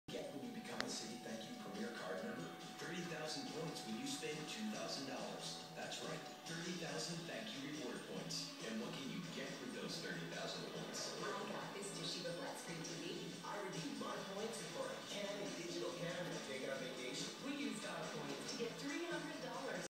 Air Piano
Aka Fluffy biscuits by FIV cat Morris. He is such a lover!  5-20-12 at Whitesboro Tx.